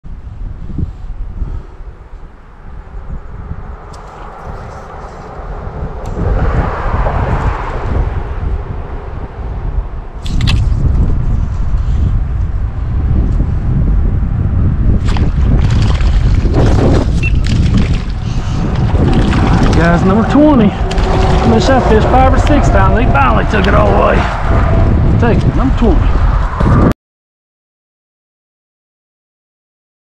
Trout slams a woolly bugger!!🎣
Trout smashing a woolly bugger on a 3wt fly rod!!

SW Virginia outdoorsman